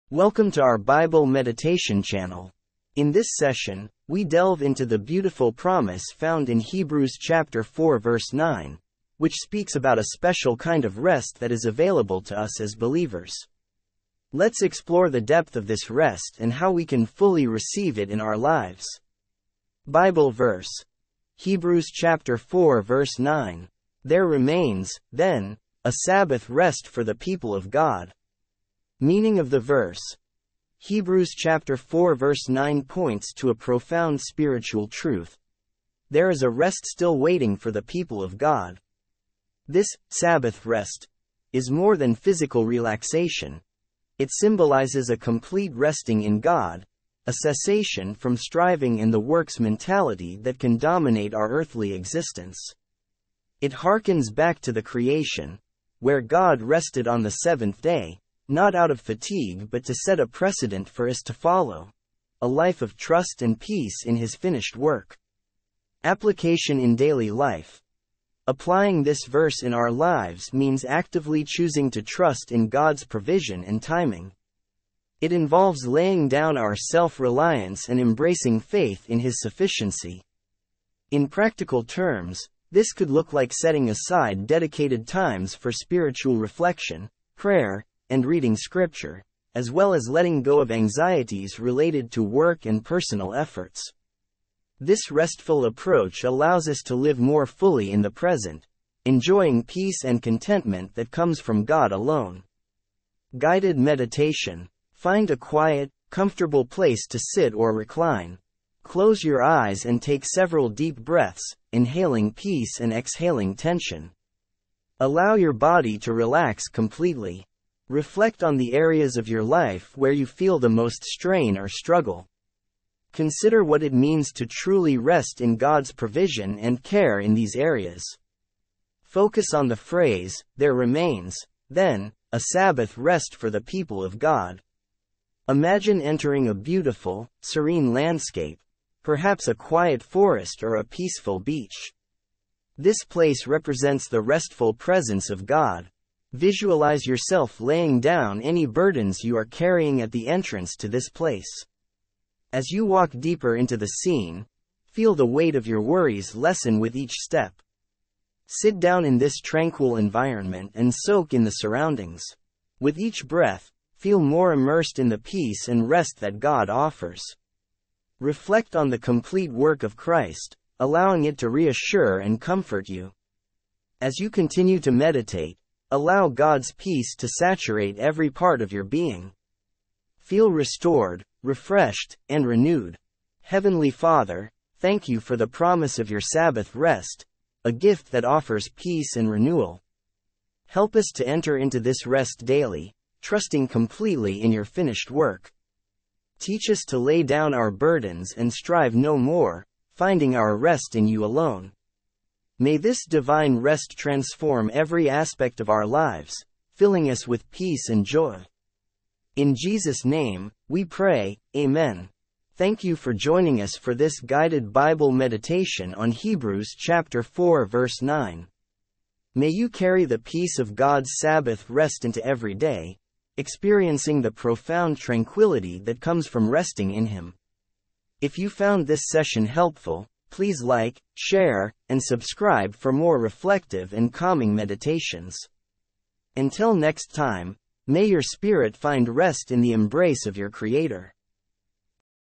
🌟 Guided Bible Meditation: Embracing Spiritual Rest in Hebrews 4:9 🙏
Join us for a  Guided Bible Meditation focused on Hebrews 4:9. This session will explore the concept of spiritual rest that God promises to His people, helping you to understand and embrace the rest found in Christ. Dive deep into tranquility as we meditate on this scripture and learn to let go of worldly burdens. 🌟🙏